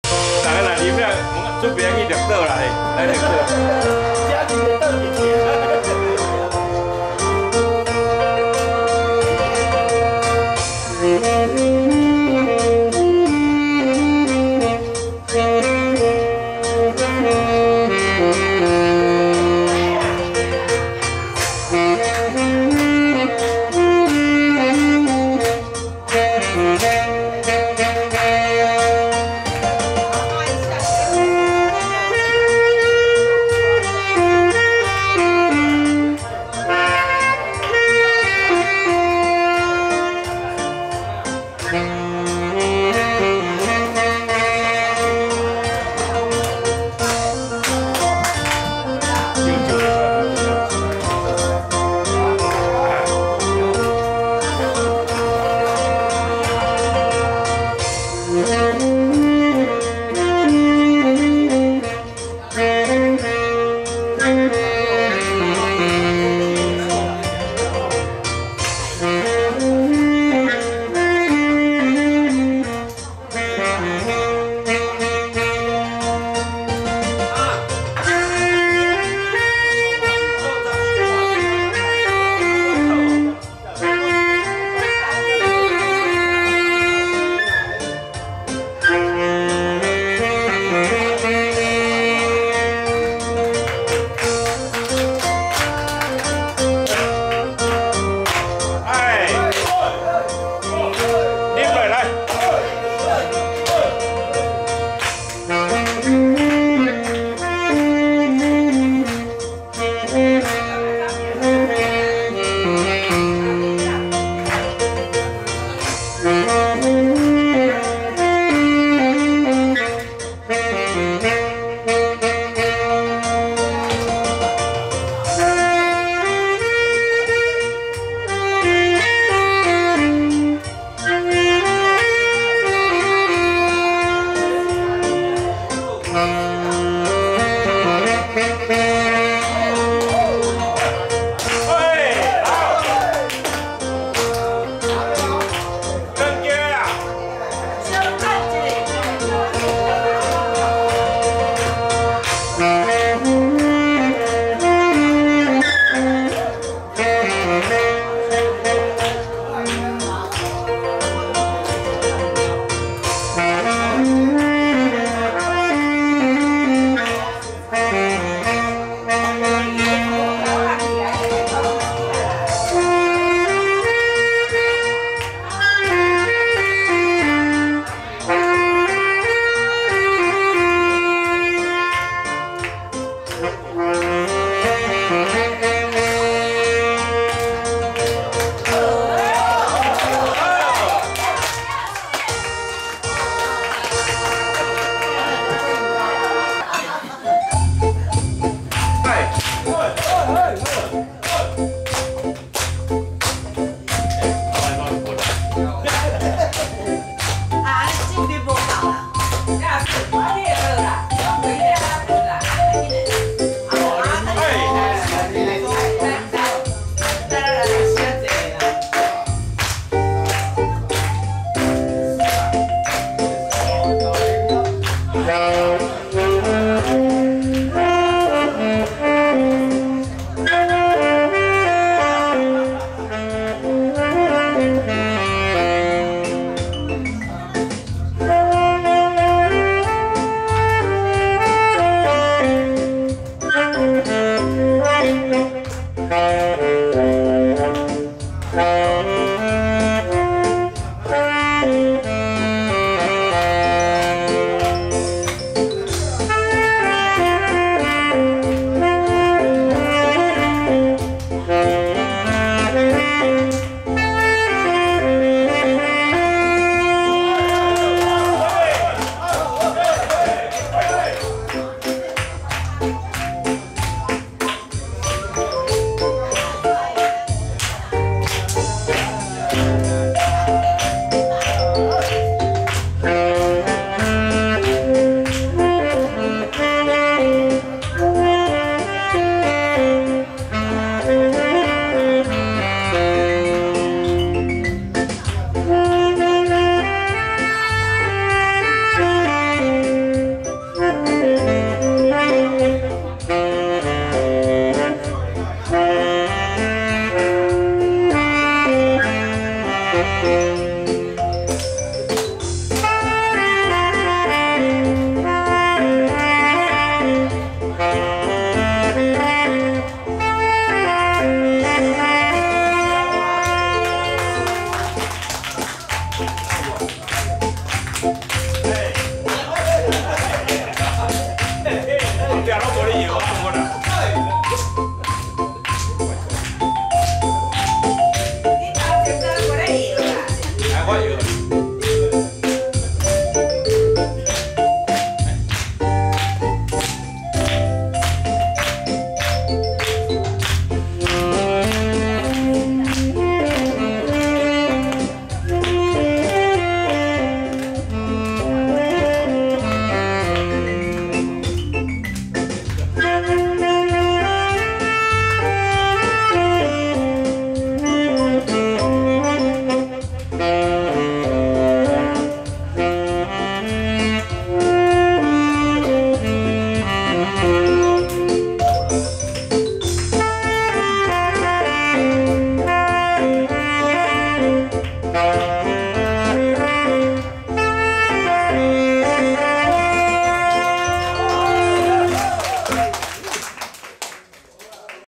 MAXXIS音樂學院
2016/12/3-4台東兩日遊.太麻里金針山.環山雅築包場演出.